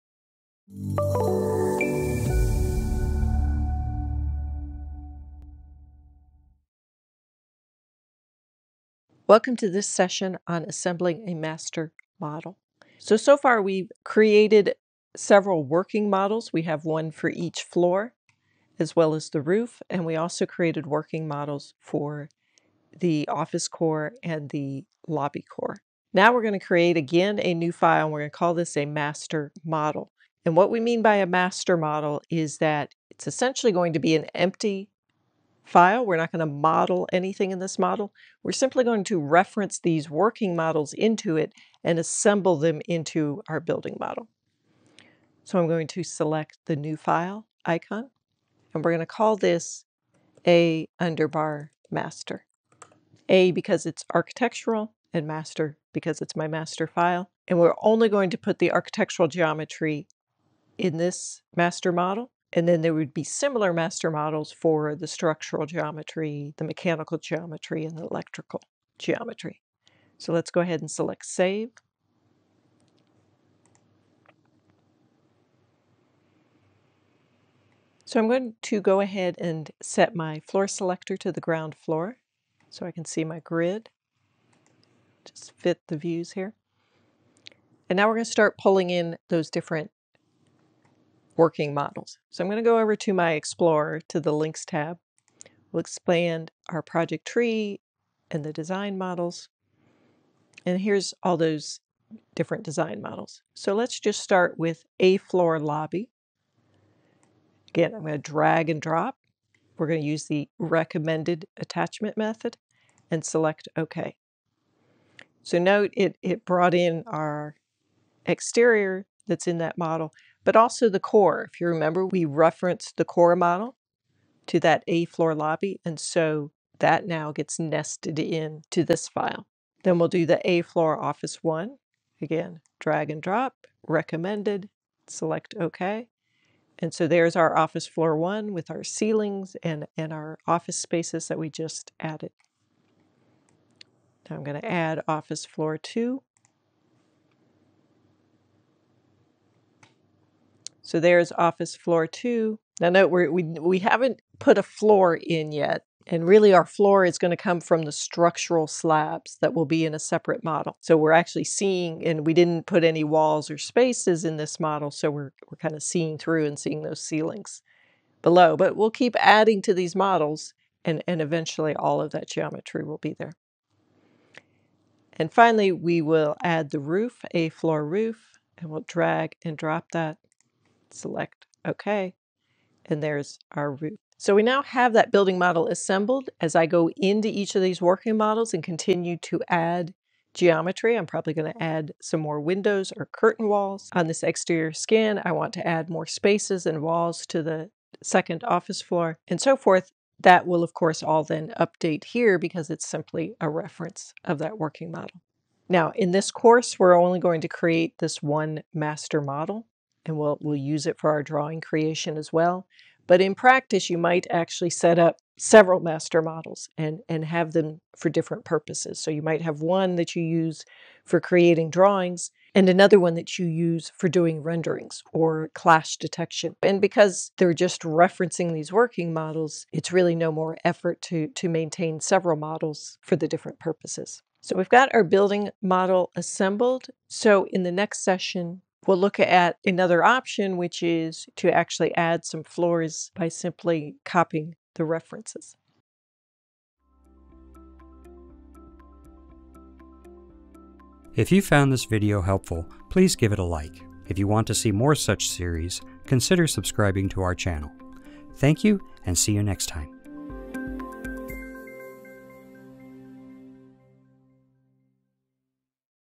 Part 8 - Assembling a Master Model in OpenBuildings Designer
In this video we will assemble all the working files to create a composition master model. To view the complete course and download a workbook visit the Bentley Learn Server.